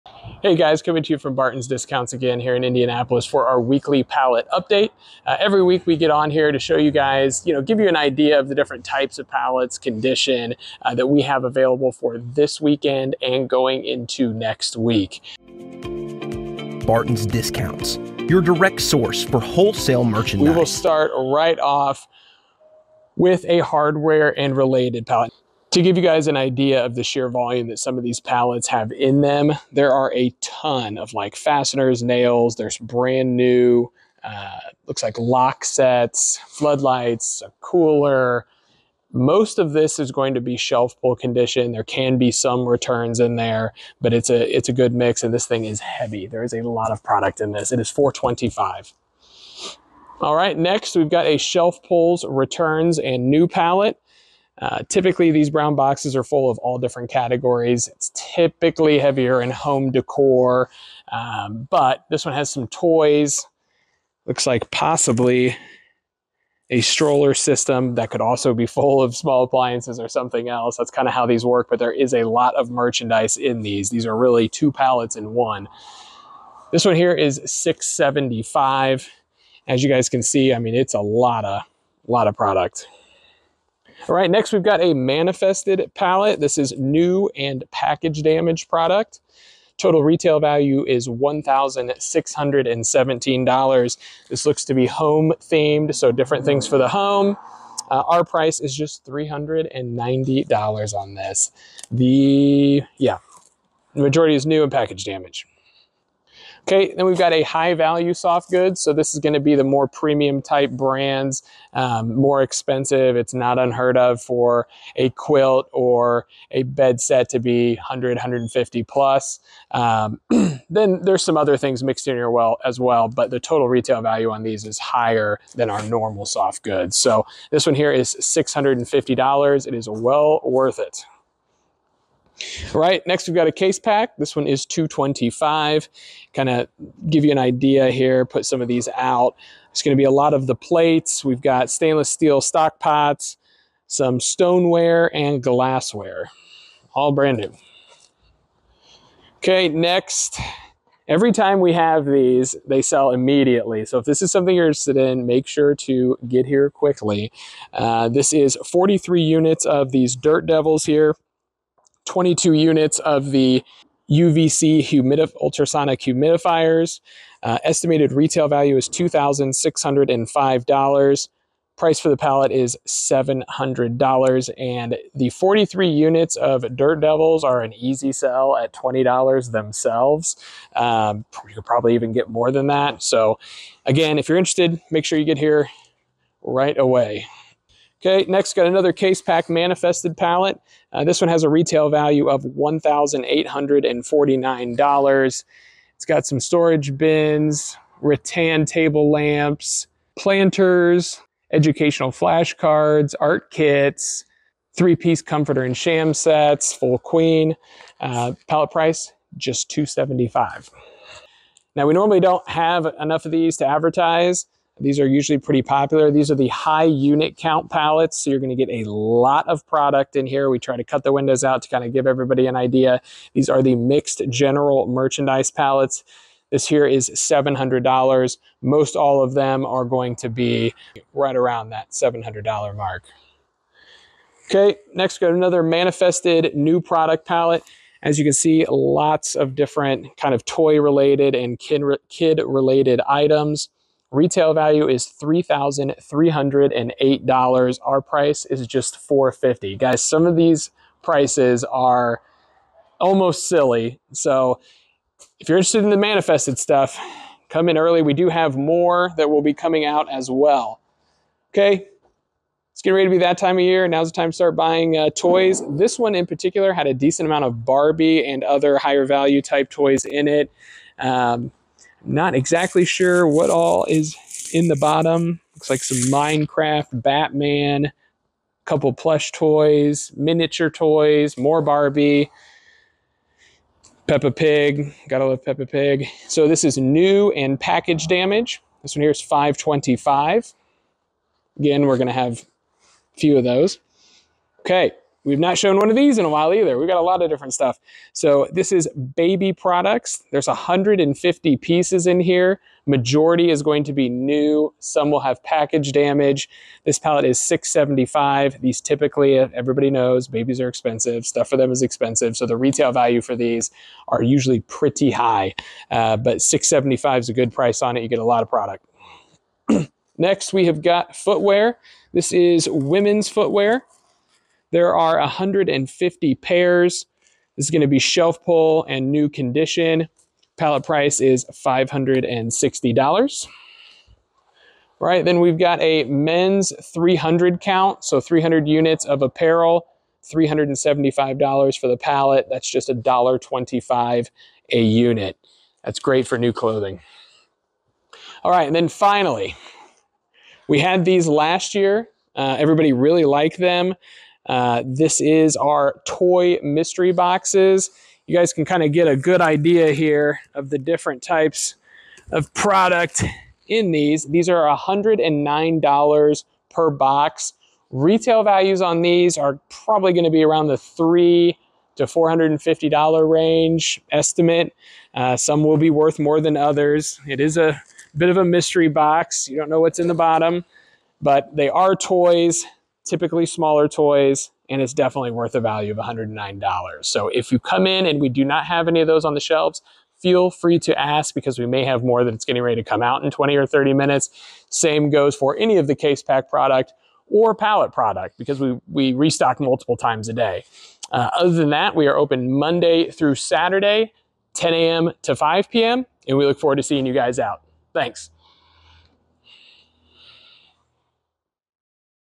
🧛‍♂️WEEKLY LIQUIDATION PALLET UPDATE🧛‍♂️ | 10/17/2024
🧛‍♂️WEEKLY LIQUIDATION PALLET UPDATE🧛‍♂️
✅ Hardware
✅ Women's Footwear
✅ Men's Apparel
✅ Toys
🍼Baby Products
And SO much more! Come see everything at 2222 Hillside Ave Indianapolis, IN 46218